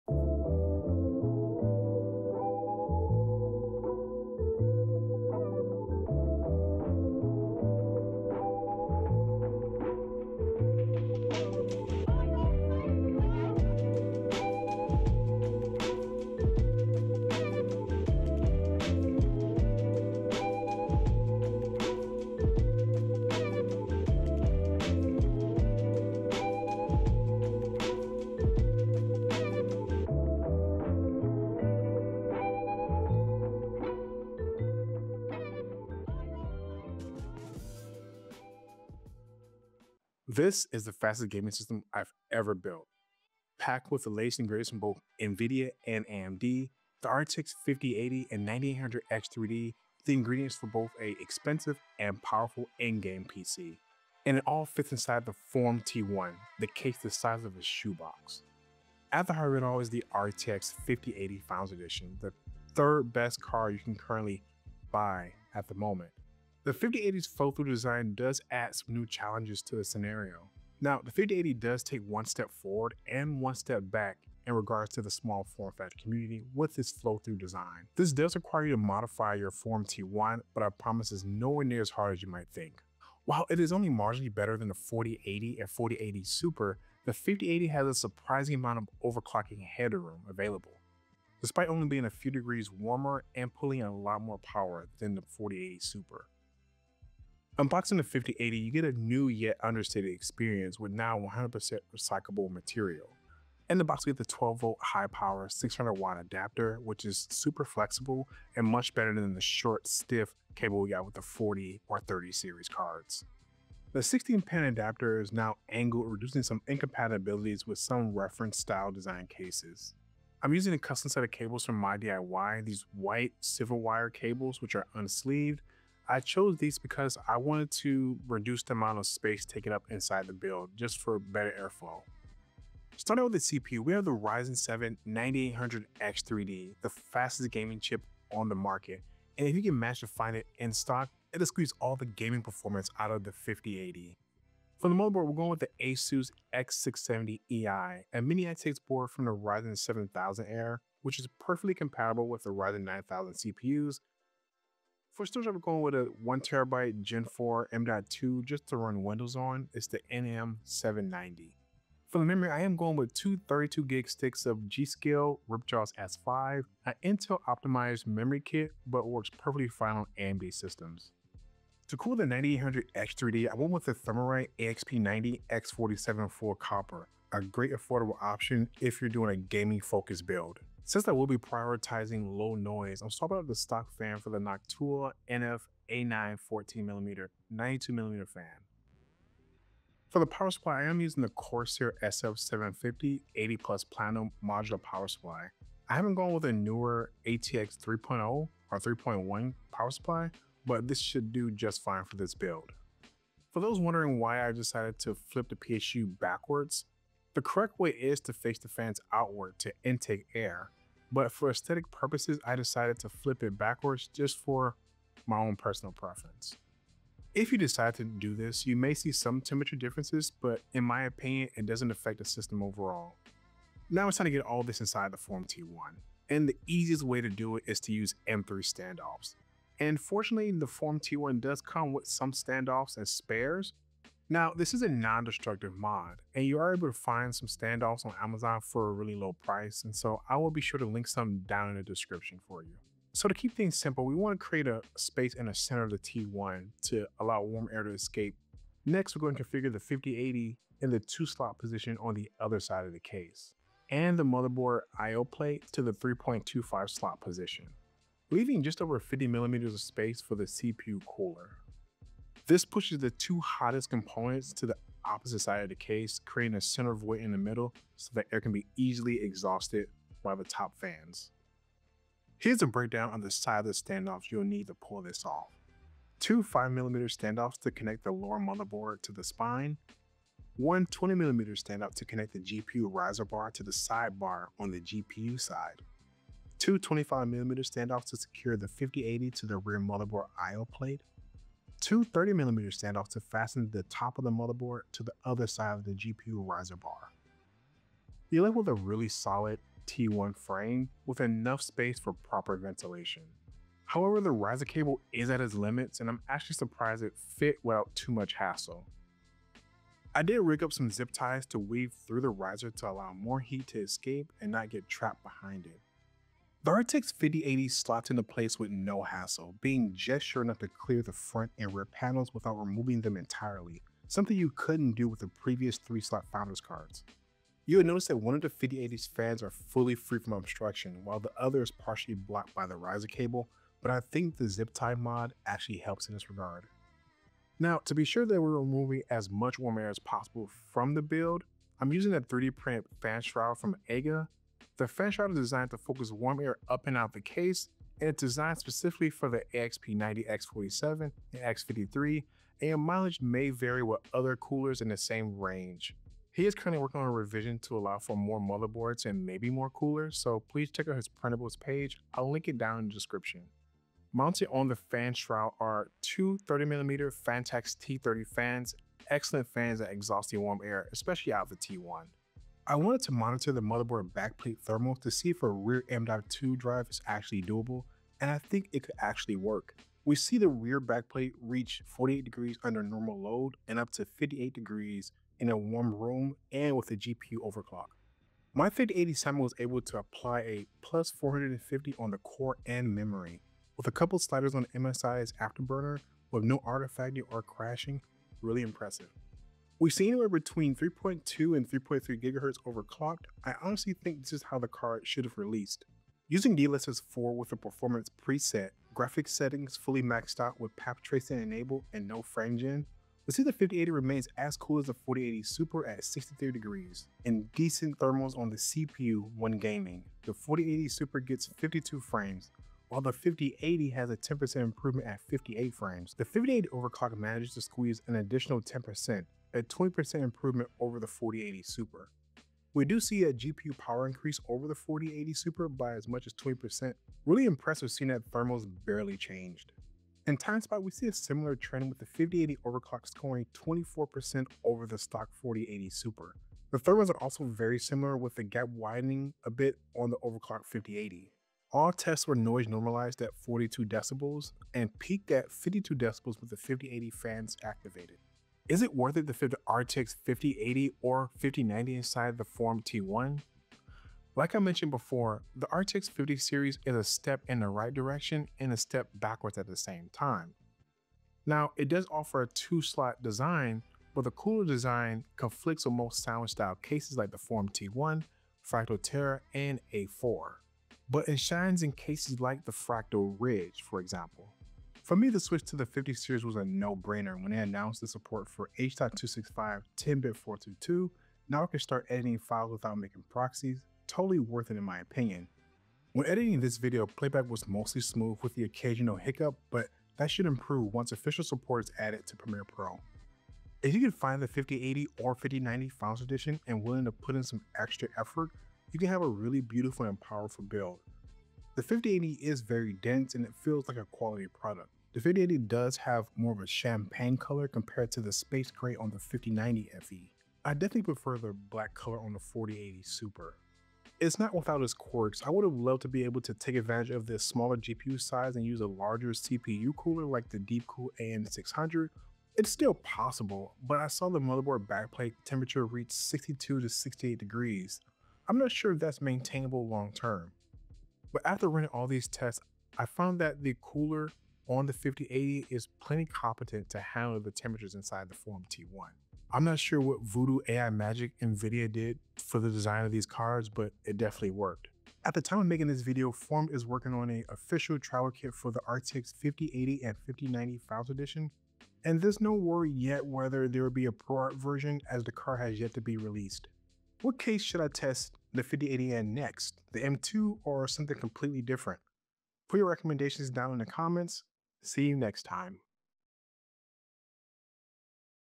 Air Optimized! RTX 5080 + Ryzen 9800X3D in the FormD T1
The FormD T1 has always been an iconic small form factor case, and today, I’m pushing it to the limits with NVIDIA’s RTX 5080 and AMD’s Ryzen 7 9800X3D. Can this ultra-compact build handle the latest high-performance hardware while keeping thermals in check? Let’s find out!

GPU: NVIDIA RTX 5080 Founders Edition
CPU: Ryzen 7 9800X3D
Motherboard: ASUS ROG Strix X670E-I
Memory: G.Skill RipJaws S5 DDR5
Cooler: Thermalright AXP-90 X47 Full Copper
Cooler Fan: Noctua NF-A9X14-HS
Case Fans: Phanteks T-30 High Performance Fans
Power Supply: Corsair SF750 80+

Mods:
Standoff Kits (6mm standoffs work in place of 5mm standoffs)
Custom 3D Printed Fan Shroud: EIGA FORMD T1 2.1 Fan Shroud (Modified)

Thermals:

Ambient Temperature: 24ºc (below temperatures are sensor readouts and not over-ambient).

T-SENSOR: Thermal sensor taped to the motherboard backplate for accurate flow-through temperatures from the RTX 5080 FE

Idle:
CPU: 44ºc
GPU: 32ºc
T-SENSOR: 37ºc

Gaming:
Cyberpunk 2077 (Synthetic):
DLSS Performance | RTX | PTX

1440P: 105 FPS
CPU TDIE: 65ºc
GPU TEMP: 58ºc
CPU PWR: 65W
GPU PWR: 260W
T-SENSOR: 48ºc

4K: 61 FPS
CPU TDIE: 60ºc
GPU TEMP: 64ºc
CPU PWR: 54W
GPU PWR: 310W
T-SENSOR: 46ºc

3DMark Time Spy Extreme: 271,126
CPU TDIE: 57ºc
GPU TEMP: 65ºc
CPU PWR: 50W
GPU PWR: 350W
T-SENSOR: 42ºc